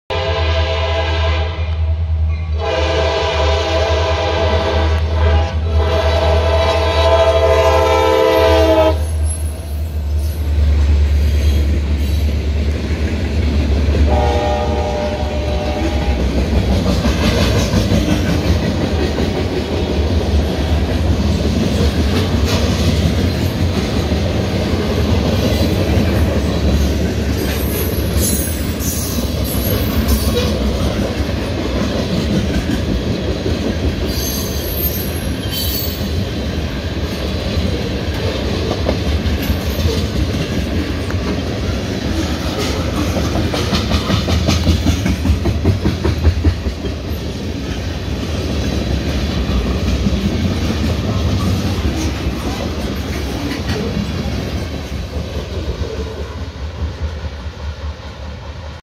CSX 6247 L399
East Morris train heads home to Ottawa with their train for some unknown reason Seneca, IL 6-9-24